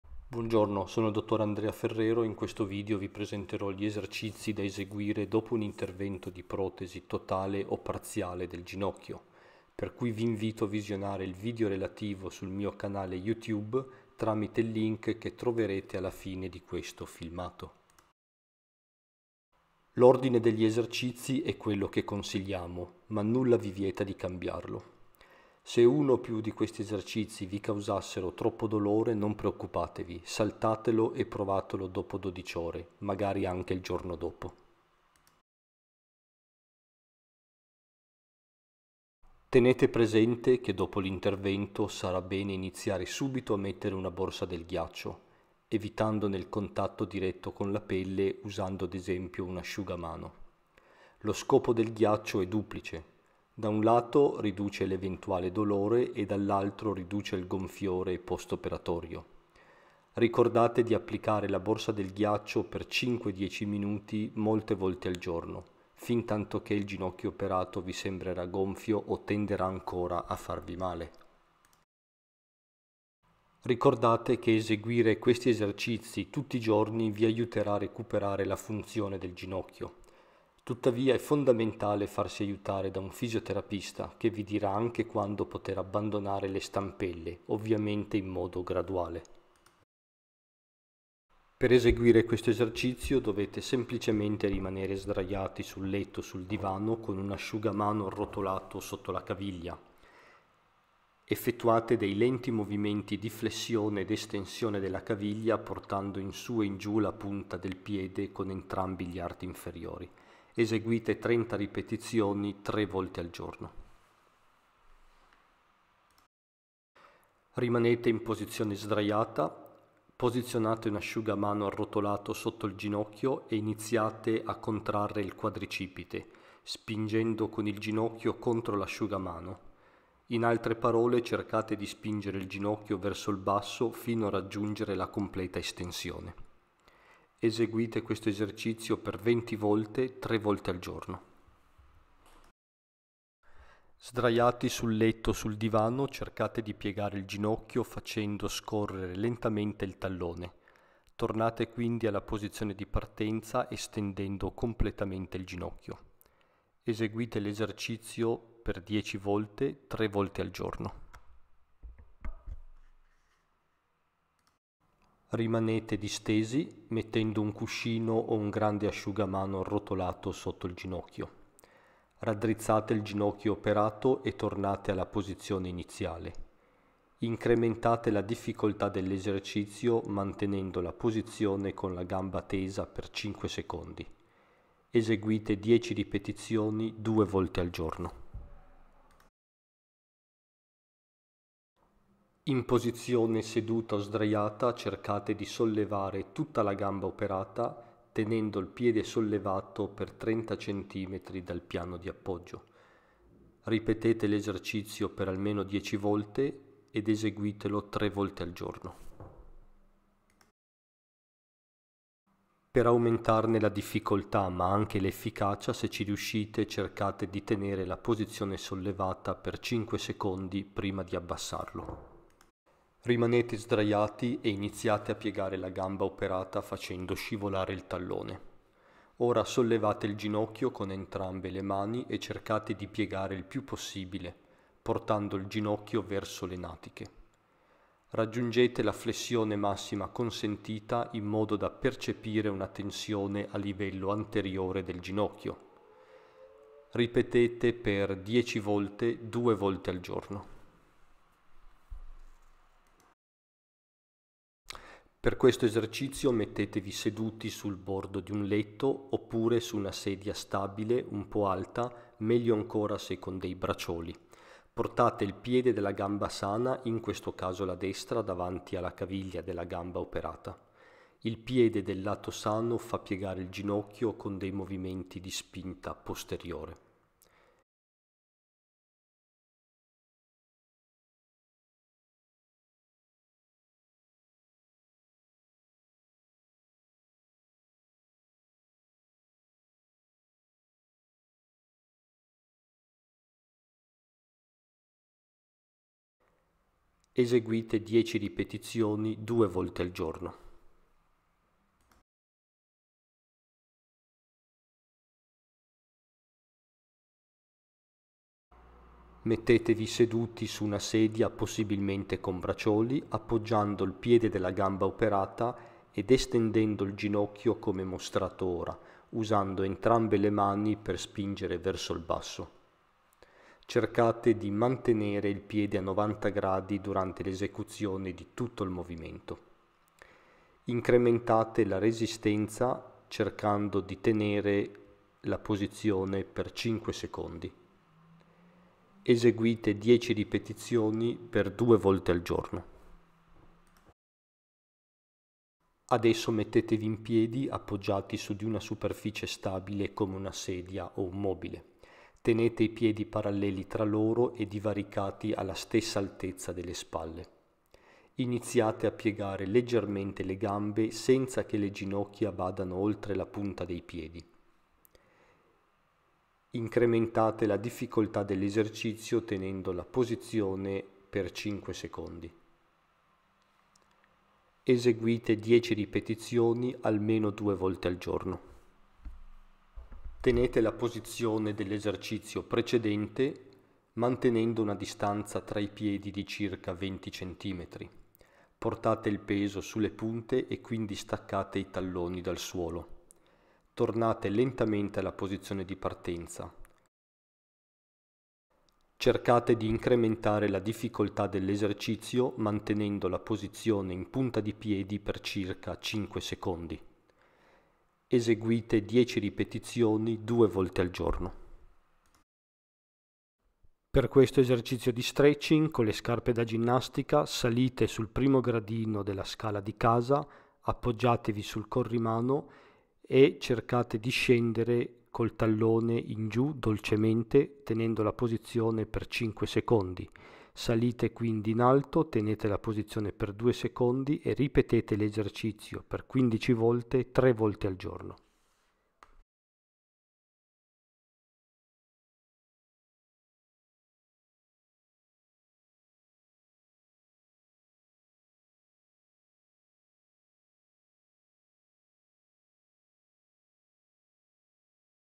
TOP 11 ESERCIZI DOPO LA PROTESI TOTALE O PARZIALE DEL GINOCCHIO [subtitles available]
In questo video vi presenterò gli esercizi da eseguire dopo un intervento di protesi totale o parziale del ginocchio, per cui vi invito a visionare il video relativo  sul mio canale YouTube tramite il link che troverete alla fine di questo filmato.
Eseguire questi esercizi tutti i giorni vi aiuterà a recuperare la funzione del ginocchio. Tuttavia è fondamentale farsi aiutare da un fisioterapista che vi dirà anche quando poter abbandonare le stampelle, ovviamente in modo graduale.